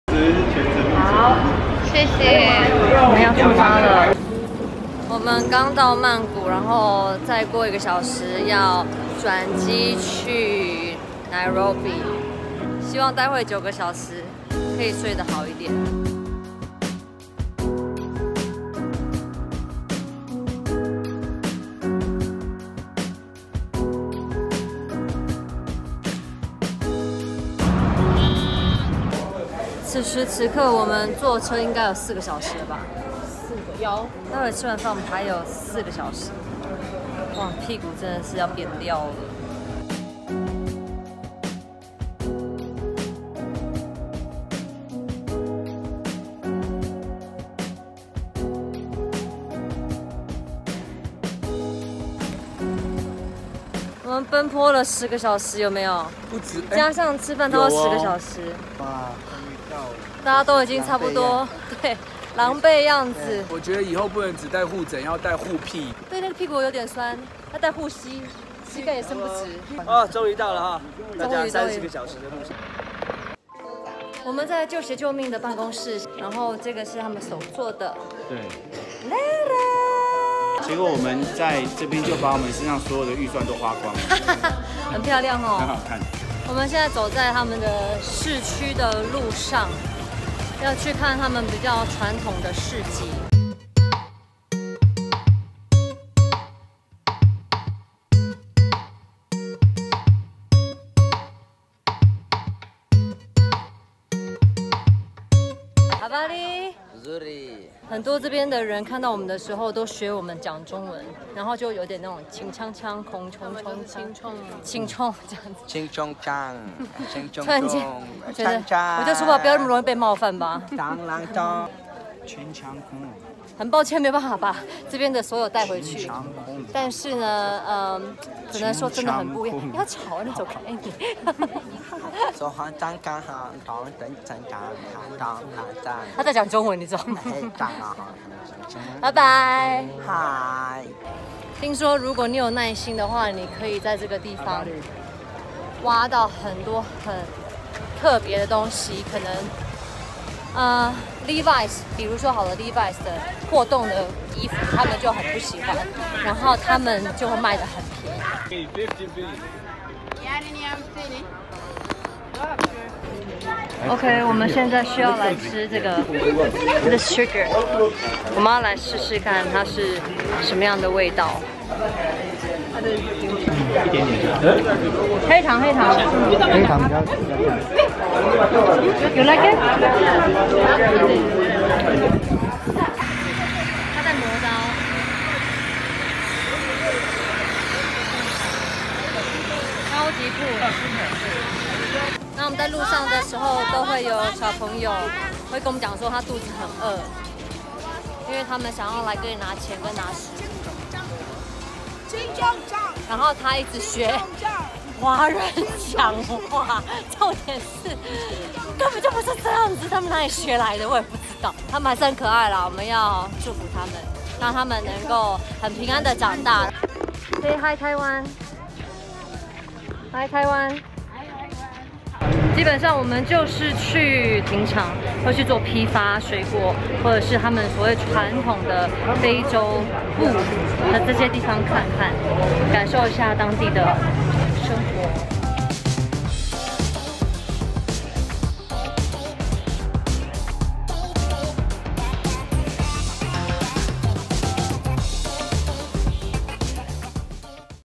我的肯亞基塔萊一遊 /舊鞋救命Step30
第一次來到肯亞坐車坐了10小時，
體驗壹下當地人的生活。
在附近的市場走一走認識這個城市，
有需要幫助的地方也有繁華的地方，

有機會你也可以跟舊鞋救命一起去當志工喔。

更多其他資訊可以到舊鞋救命的官網上去了解一下。